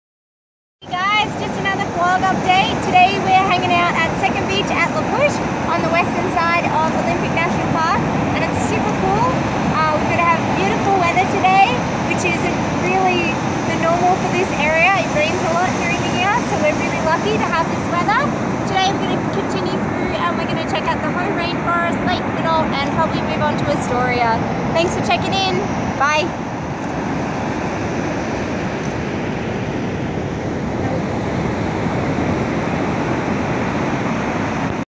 Vlog update La Push beach
Nicole nerding out as we visit various twilight settings today